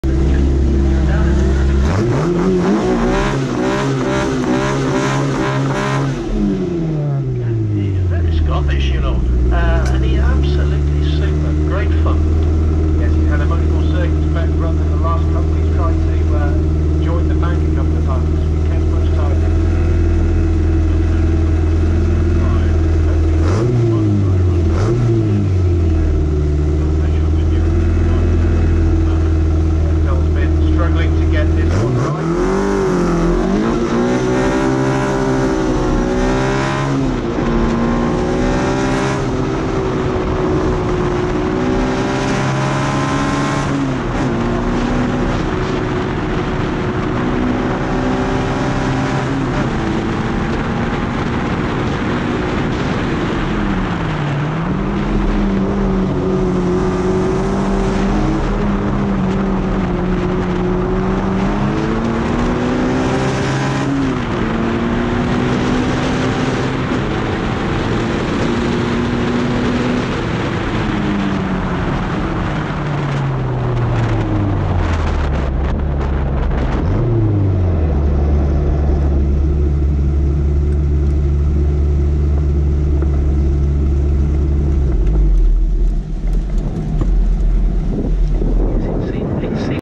Shelsley Walsh Classic Nostalgia 29 July 2018 1971 Mallock MK11B
Philip Stader driving a 1971 Mallock MK11B in the Bugatti Owners Club Aldon Automotive Classic Hill Climb Championship 2018 Round 7